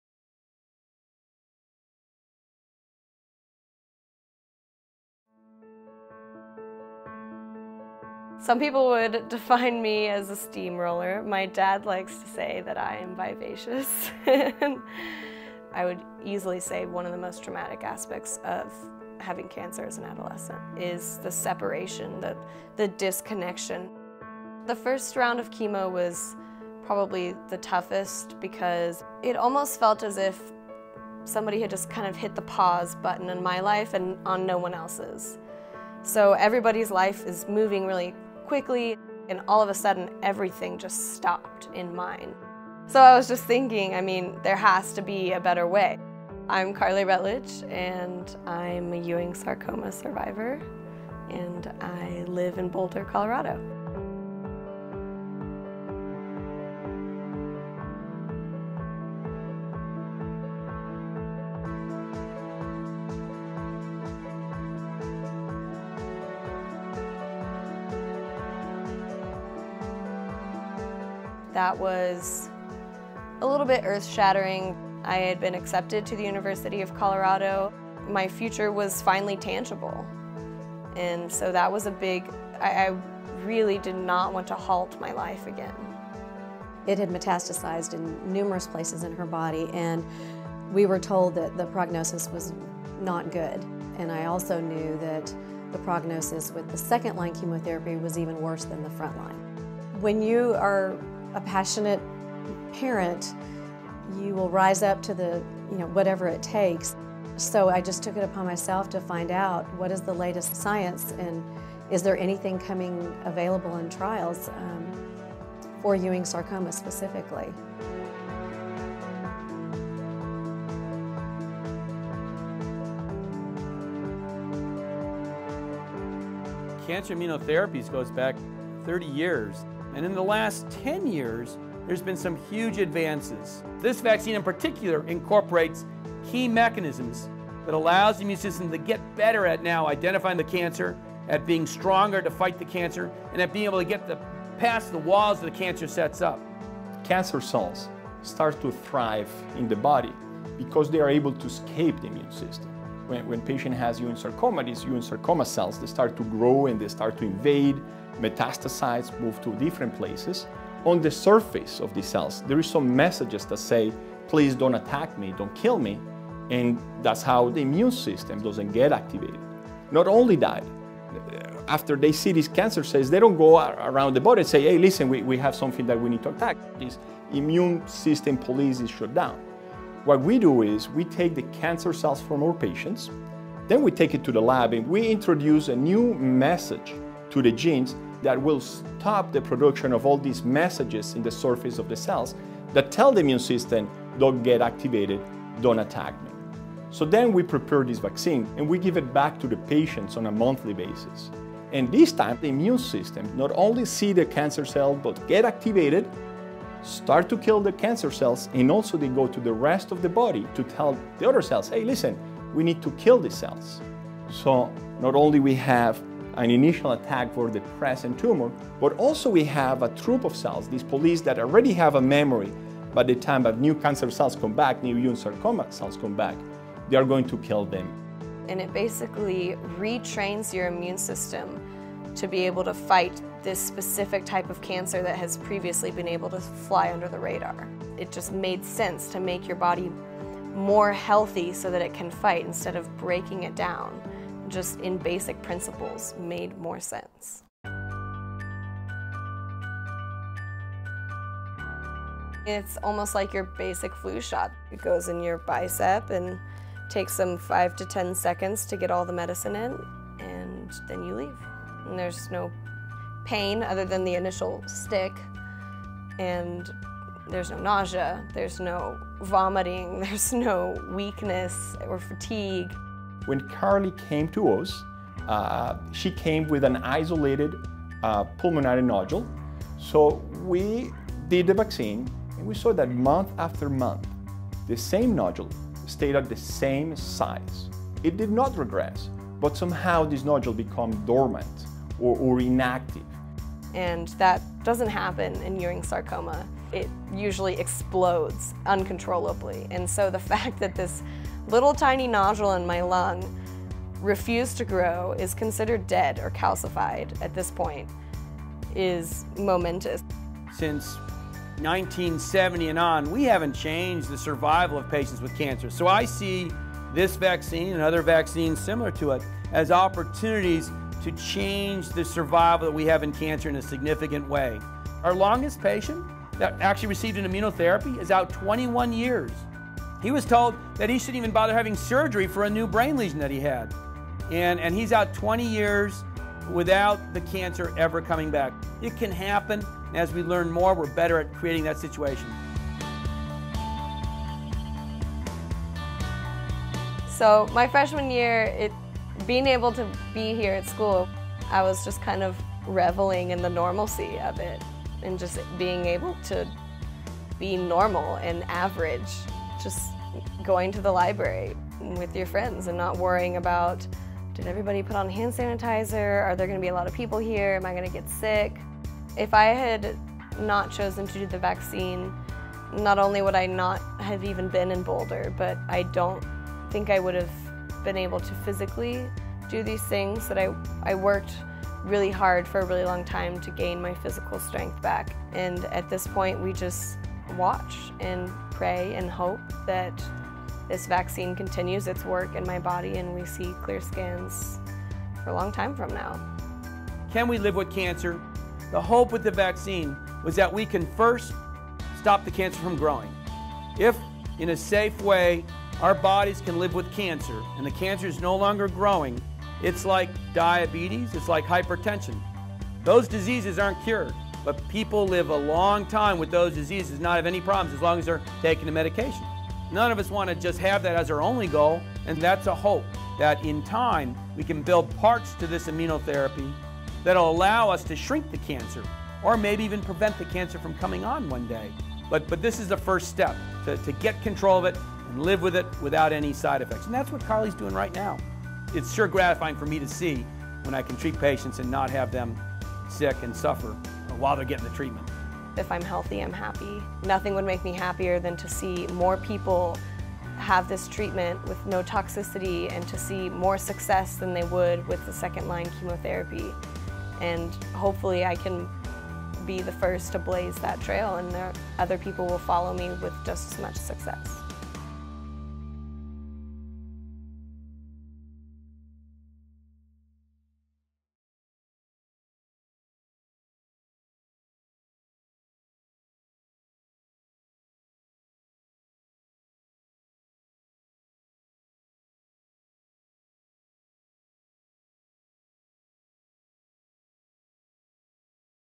Stronger, not sicker
Carley Rutledge is studying biology at university in Colorado. She loves hiking and hanging out with friends. Many of us take these things for granted, but for Carley they are only possible because she's taking part in a trial for a cancer vaccine. Without the trial, Carley would have been stuck in hospital having a second round of chemotherapy. Carley tells her story in this film. We also hear from her doctors, who are pioneering this new personalized cancer treatment that uses a patient's own immune system to fight the disease.